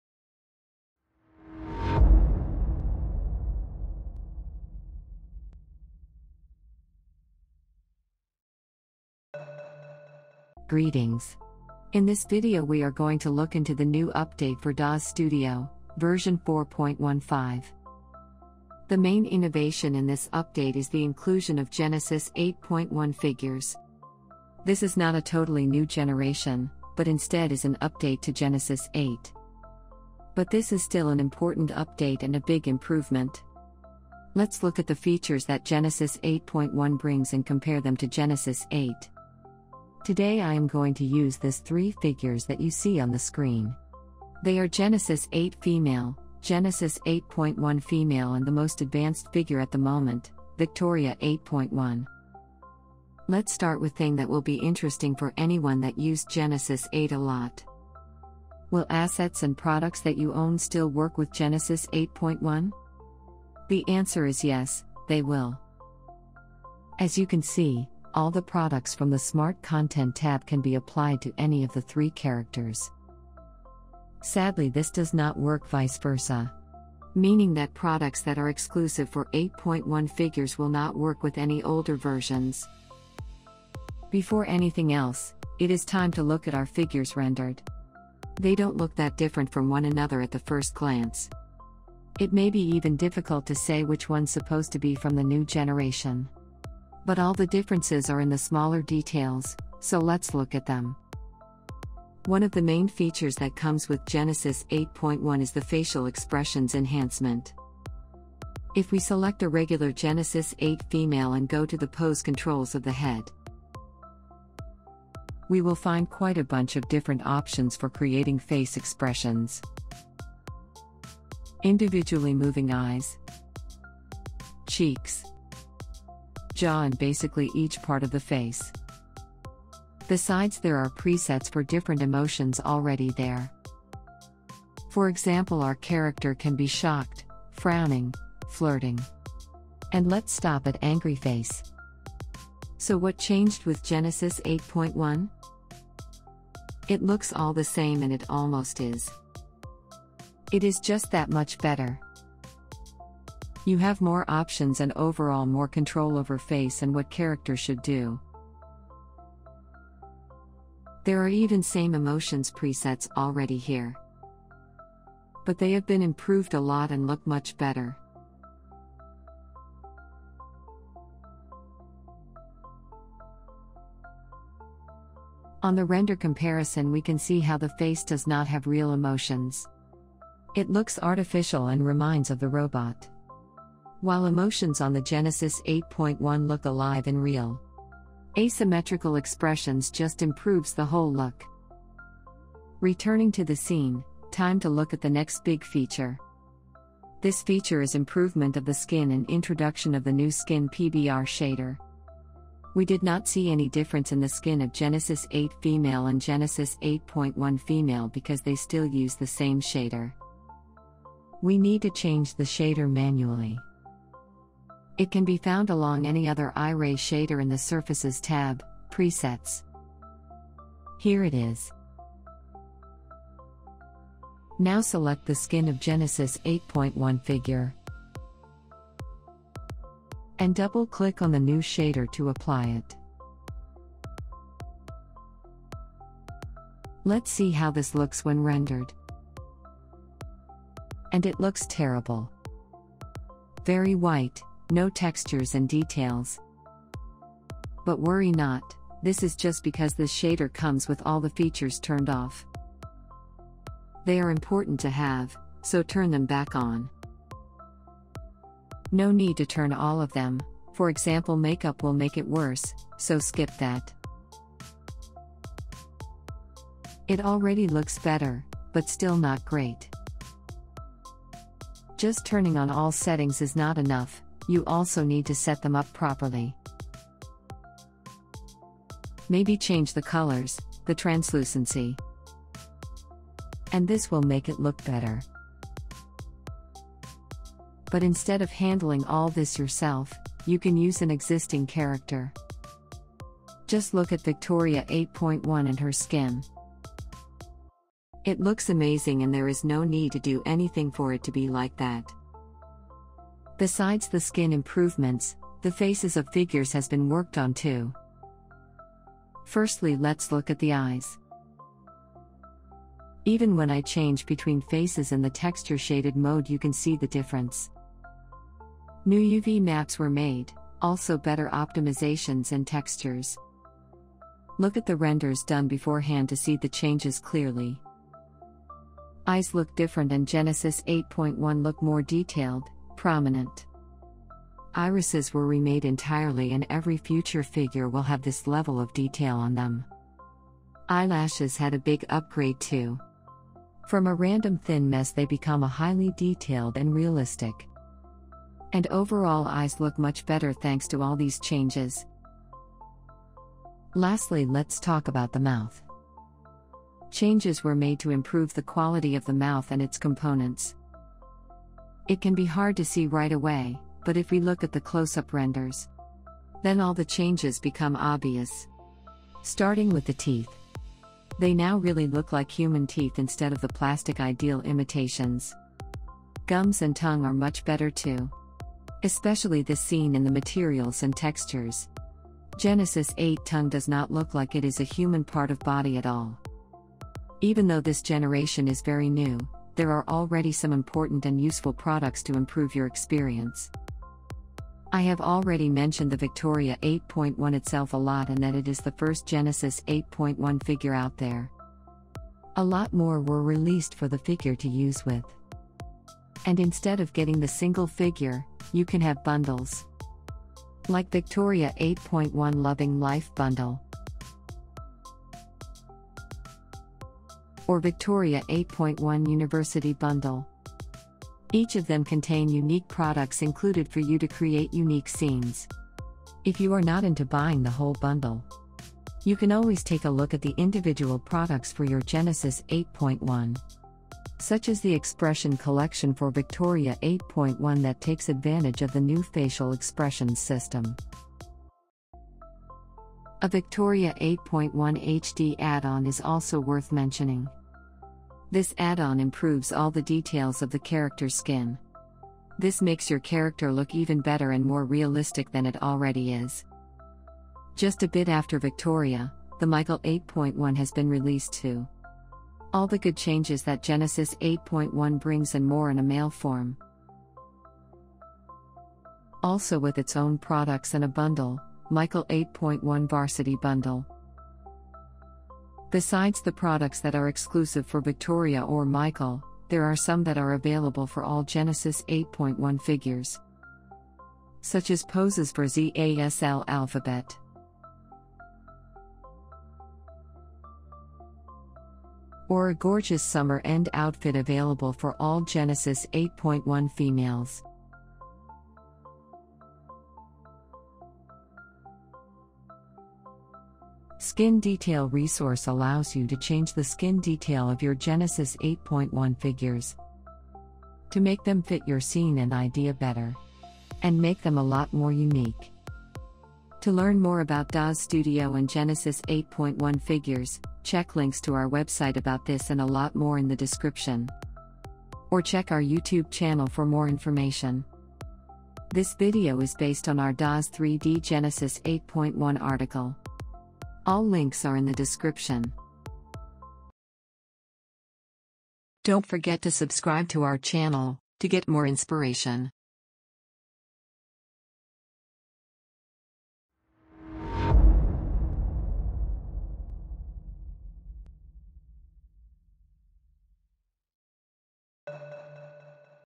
Daz3D Genesis 8.1 Overview & Features
This video highlights and compare the new Daz3D Genesis 8.1 technology with the previous generation. Links can be found in our blog post

00:00 - Intro
00:52 - Compatibility
01:23 - Initial Render
01:41 - Facial Expressions
03:12 - Skin and Shaders
05:03 - Eyes and Eyelashes
05:53 - Mouth
06:26 - Products Suggestions
08:36 - Outro